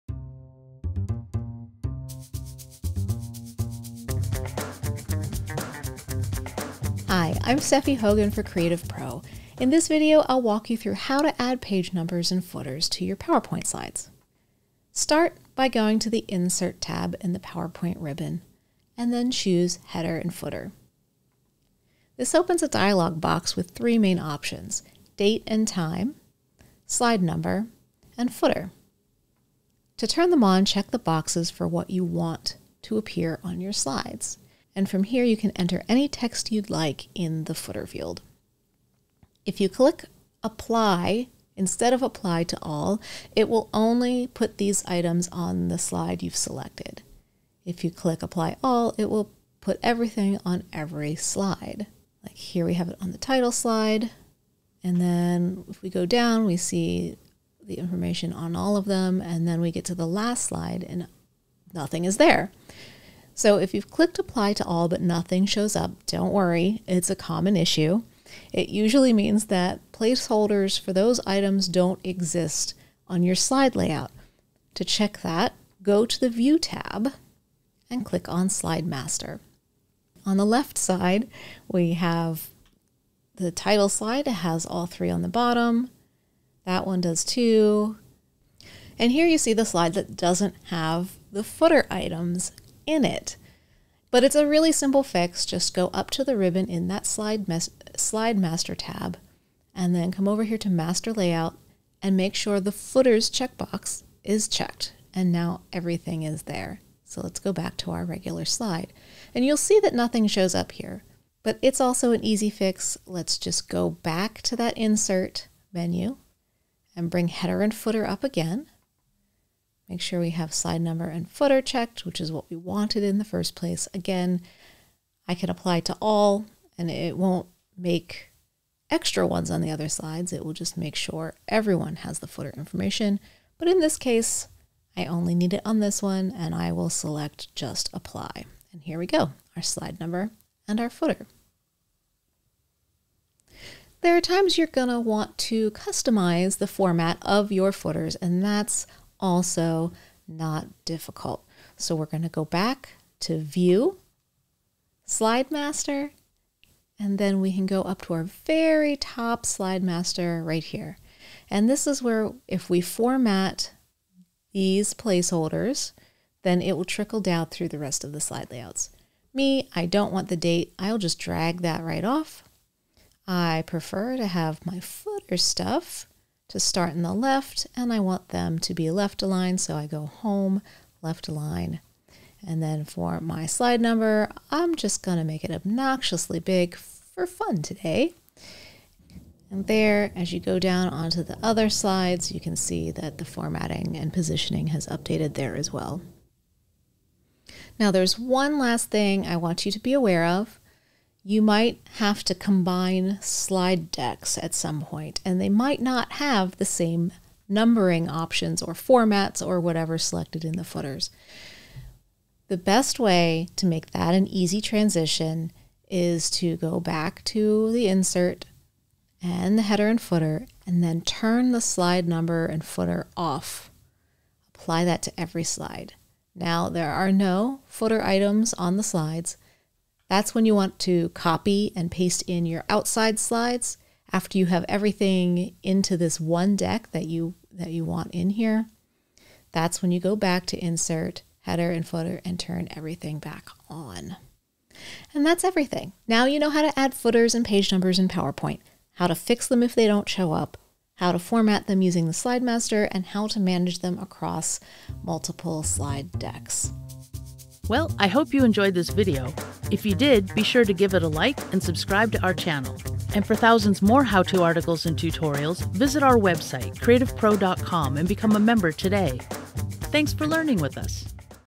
PowerPoint: How to Add Slide Numbers and Footers (Video Tutorial)
In this PowerPoint how-to video, Stephy Hogan shows how to add slide numbers and footers. She demonstrates how to customize the info and to apply to just the current slide or across all slides. Lastly, Stephy explains how to fix the footers and page numbers if they don’t show up as well as how to apply them across multiple slide decks.